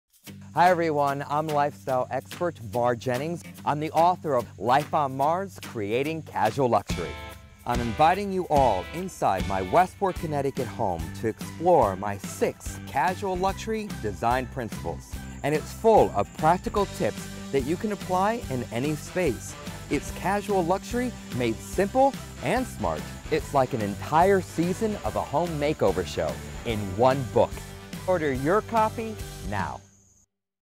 Mar Jennings' LIFE ON MAR'S: Creating Casual Luxury Commercial Spot
Emmy-nominated TV Host, Lifestyle Expert and best selling author Mar Jennings is a master at creating a casual luxury lifestyle made simple and sMARt—and making it quickly understood and easy-to-apply with his trademark sMARt tips and techniques. Mar's inventive and original blueprint for design infuses Mother Nature's colors and textures throughout the home and garden.  As one of the top lifestyle experts, Mar has been featured on HGTV, the Today Show, QVC, and in the New York Times. He is the author of the best selling Life On Mar's: A Four Season Garden, a nationally acclaimed gardening book, sold world wide.

Mar Jennings' signature style is called "Casual Luxury" and for the first time he shares how it can be broken down into six design principles that anyone can apply. Jennings' new book will continue the series with LIFE ON MAR'S, Creating Casual Luxury.

This deluxe oversized coffee table book will be filled with photographic essays—beautifully shot by renowned photographer Stacy Bass—highlighting his philosophy, plus practical applications and resourcefulness applicable for any home or space. Like an entire season of a great design show, LIFE ON MAR'S, Creating Casual Luxury will allow readers access to an encyclopedia of great ideas to apply in their home. This lifestyle expert does it with simplicity and style, balancing natural solutions with everyday elegance.